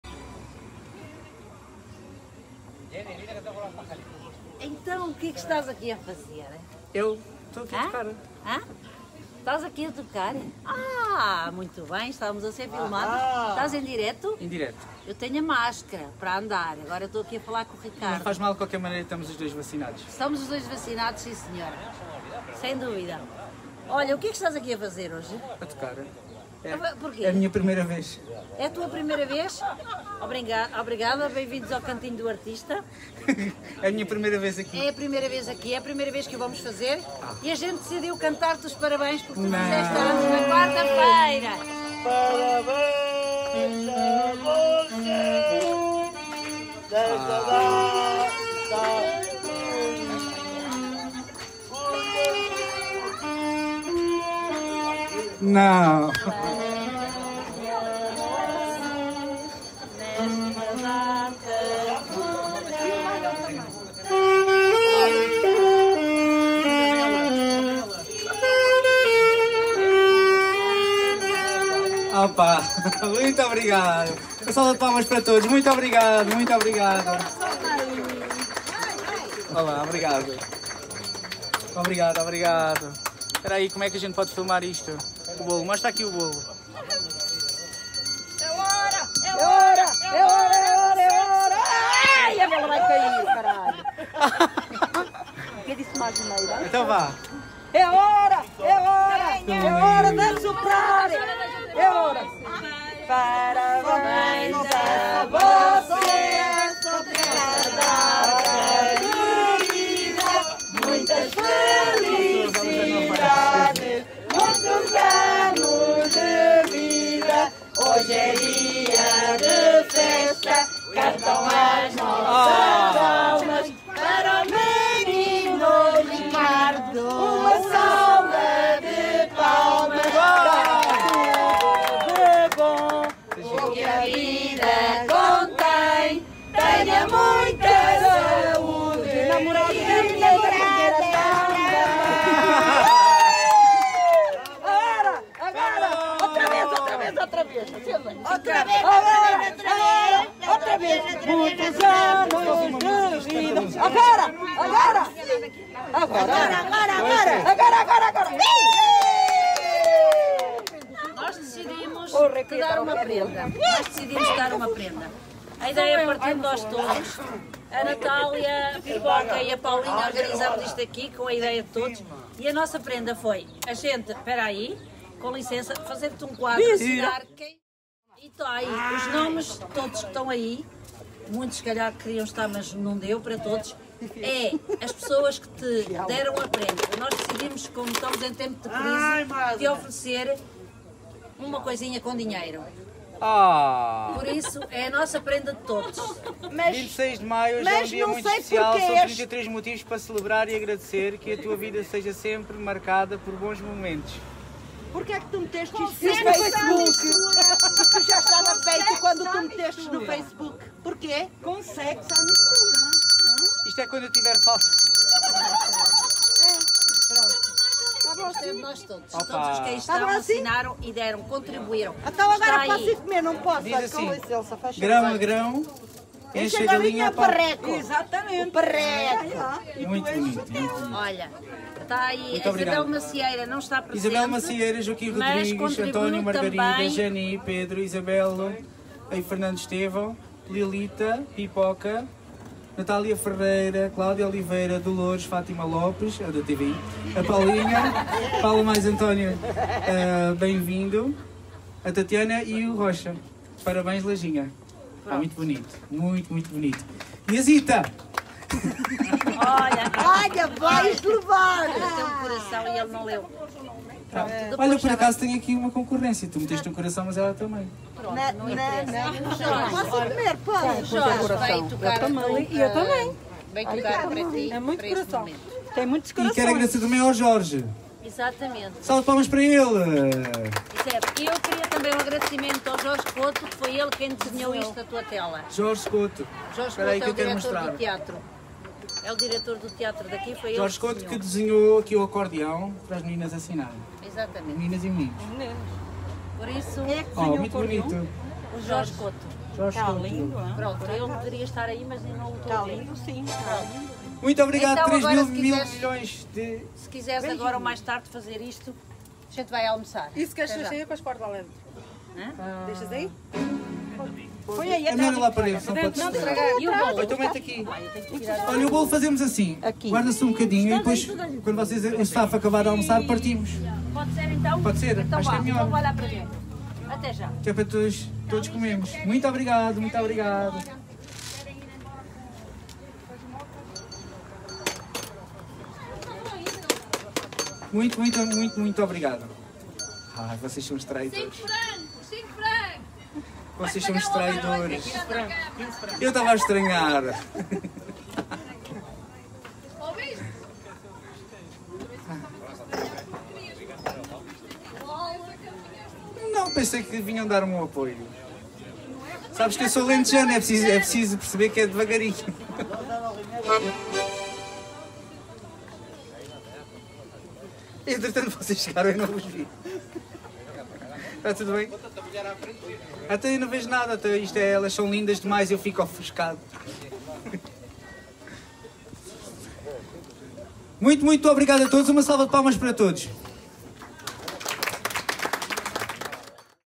Amigos surpreendem Laginha no seu 33º aniversário - Centro D. Olímpia - Uffikon, Luzern - Suíça 🇨🇭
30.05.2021 - Centro Português D. Olímpia - Uffikon, Luzern - Suíça 🇨🇭